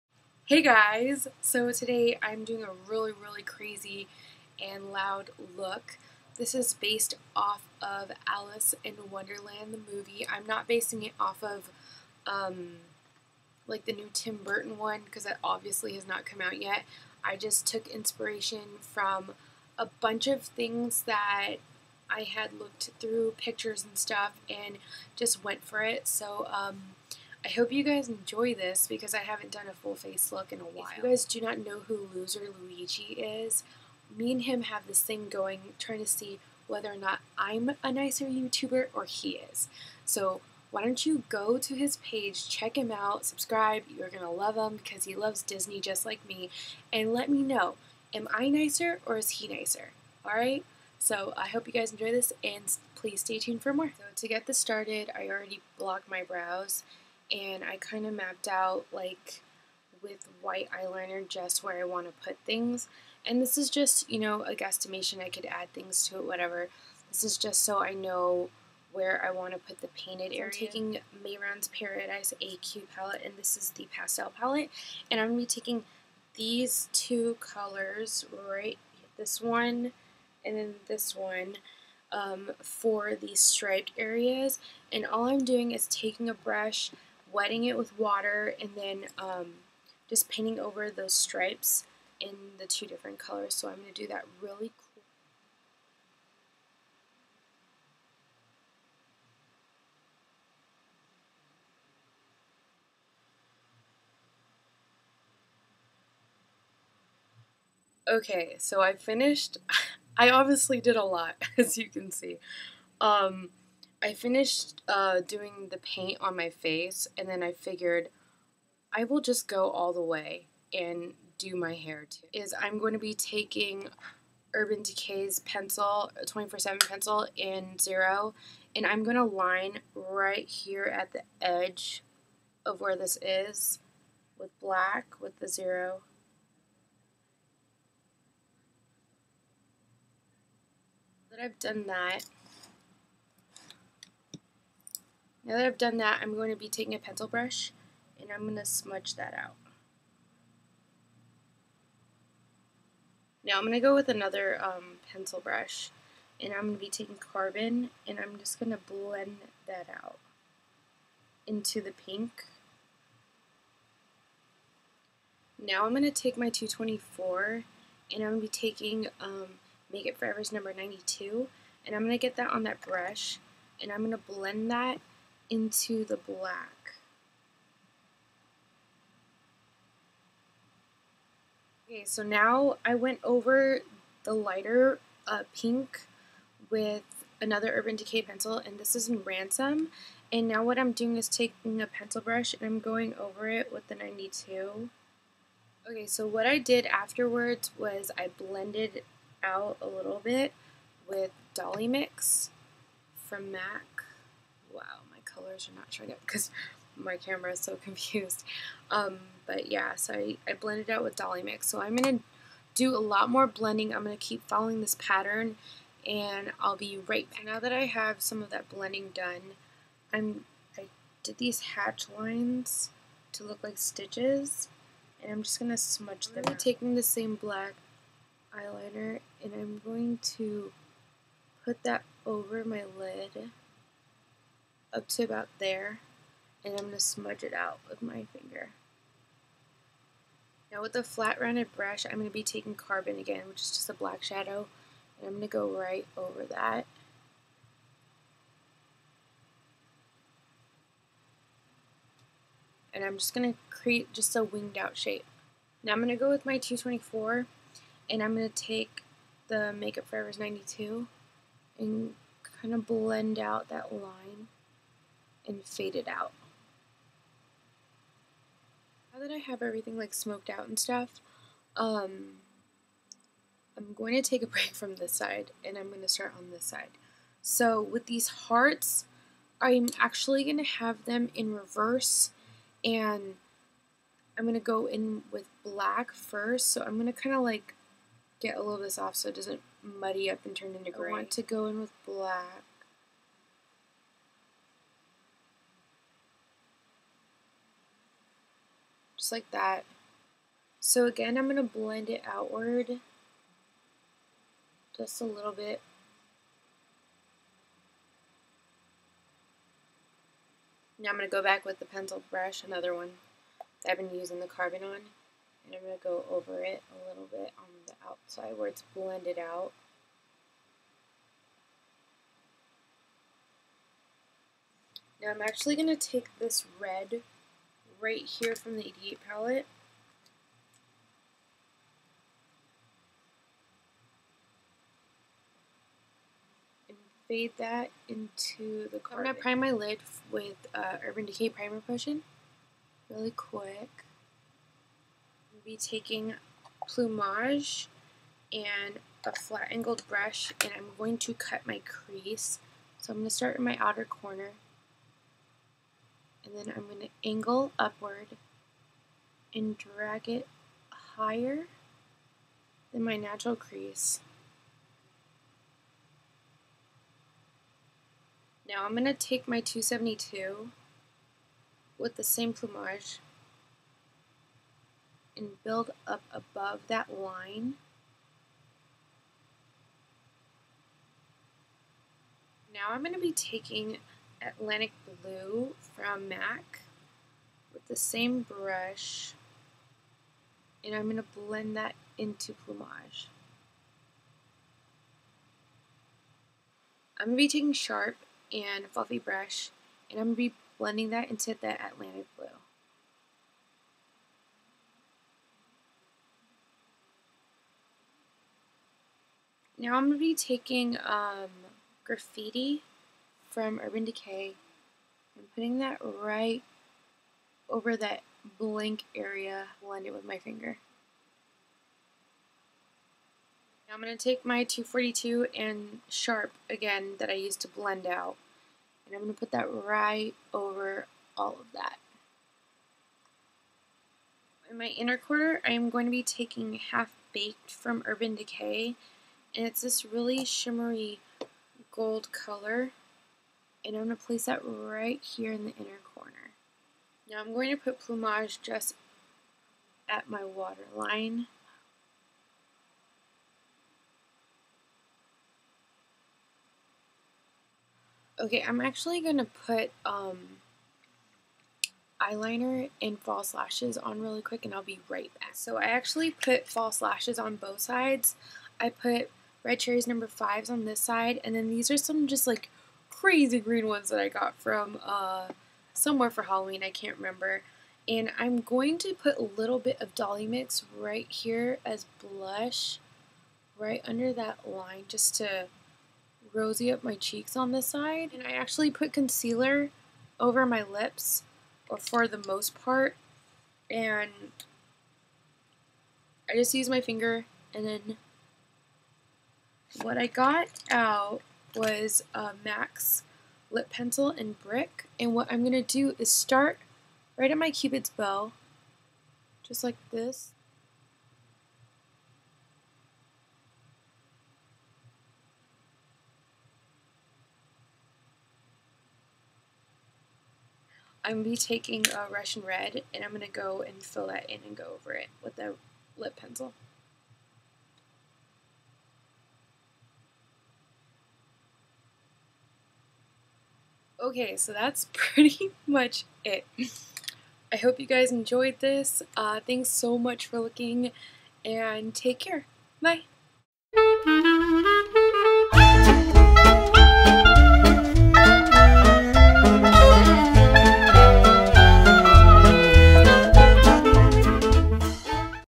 Alice in Wonderland Tutorial
This look is inspired by Disney's Alice in Wonderland released in 1951 :)

Okay, let me know, am I a nicer youtuber or loserluigi!?

Products:
Elmer's Glue Stick
MUFE Camouflage Concealer Palette 1
Clinique Translucent Powder 2
Rimmel Eye Khol in White
Mehron Paradise AQ Pastel Palette
Urban Decay 24/7 Glide On Pencil in Zero
MAC E/S in Carbon
MUFE E/S #92
Urban Decay 24/7 Glide On Pencil in Ransom
MAC Blush in Dollymix
Urban Decay Primer Potion
MAC E/S in Plumage
MAC E/S in Atlantic Blue
MAC E/S in Sharp
L'Oreal Lineur Intense Liquid Eyeliner in Carbon Black
Red Cherry Lashes #5
Green False Lashes
MAC Lip Pencil in Brick
MAC Lipstick in Russian Red